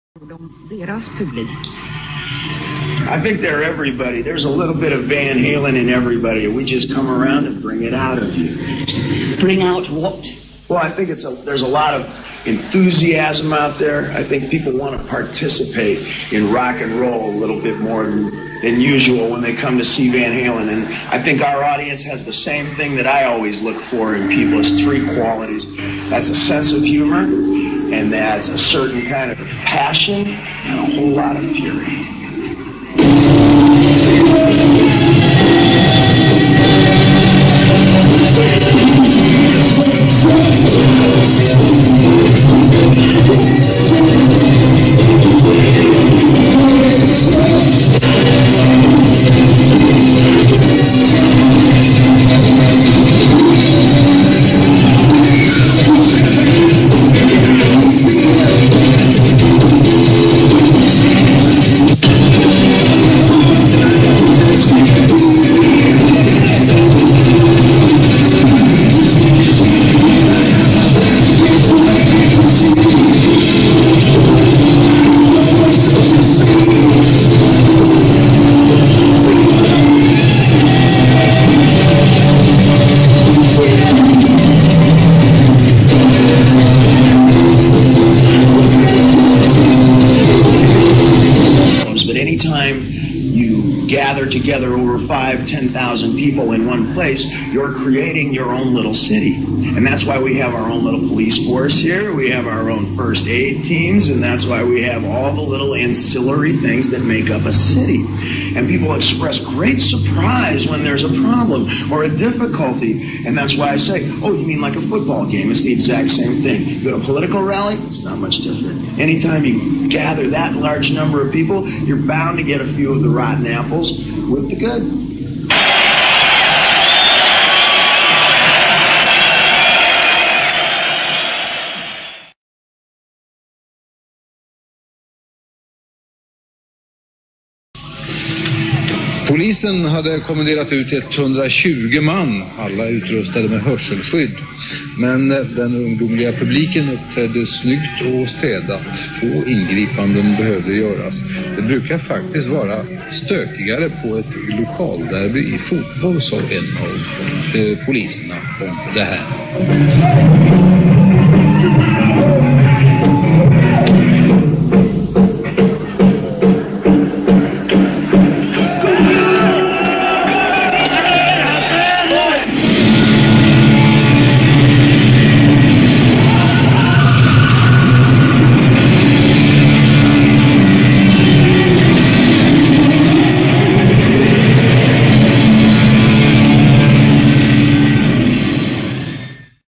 Van Halen - News Reports on Monsters Of Rock Shows Aug - Sept 1984
PAL sourced news reports on the following 2 shows:
1. Swedish TV News Report - Aug 25, 1984
2. German TV News Report - Sept 2, 1984